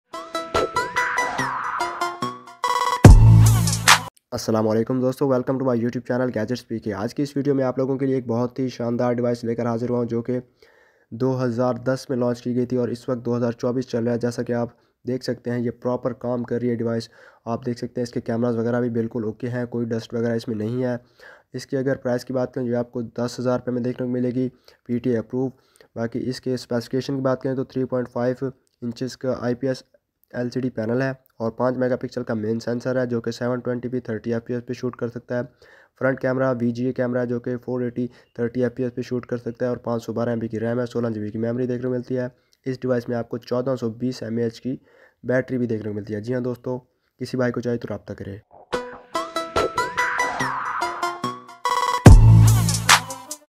iPhone 4 & 5 Review: 14 Years Later—Still Worth It? | PTA Approved!
Dive into our in-depth review of the iPhone 4 and 5, 14 years after their launch! We explore their performance, design, and functionality in today's tech landscape. Are these iconic devices still worth your time and money? Discover the pros and cons, and see if they’re PTA approved for your needs!